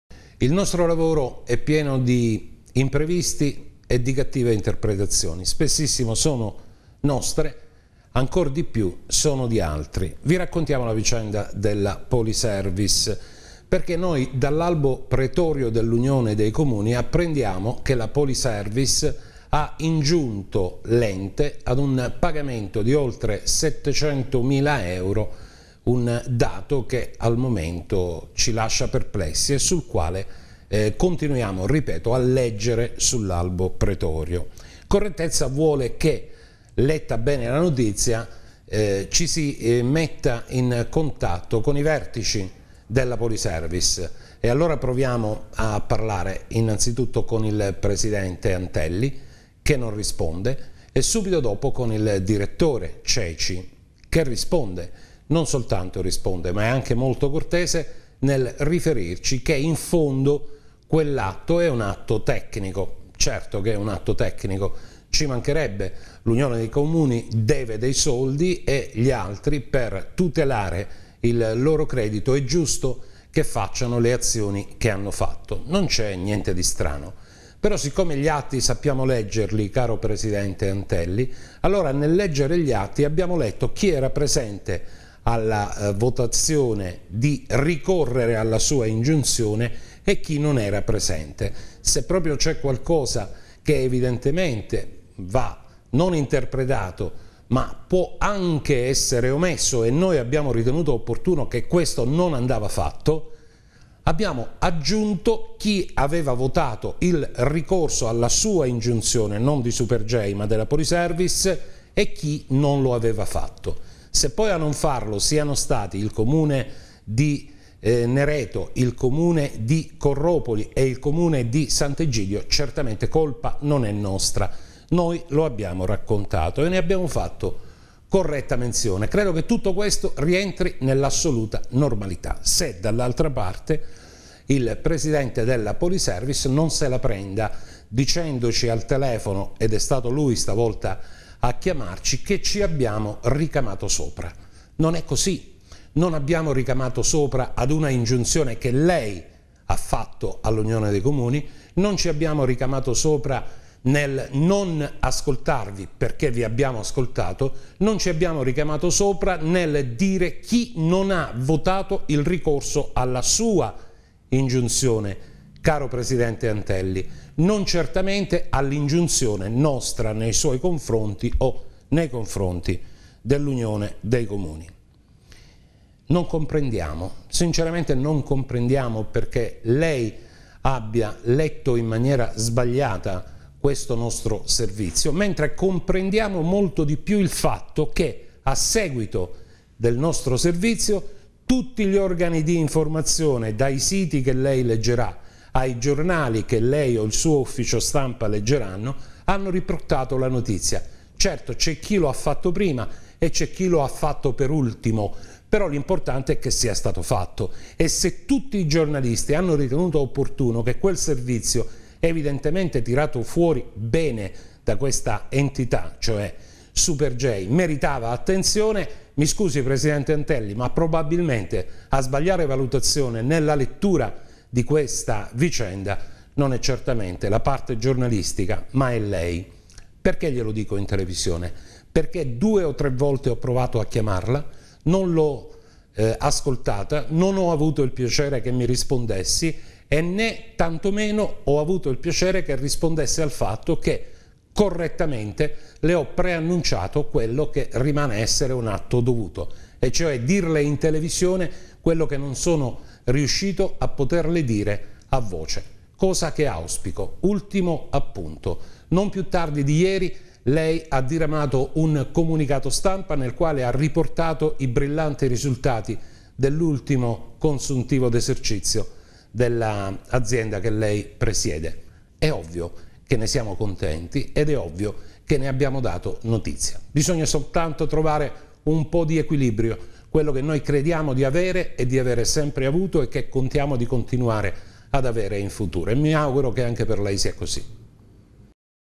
NASCE UNA "VERTENZA" SULL'INGIUNZIONE POLISERVICE
Il Direttore di Super J ritiene giusto precisare  che i termini dell'ingiunzione della Poliservice all'Unione dei Comuni della Val Vibrata restano un "atto giornalistico"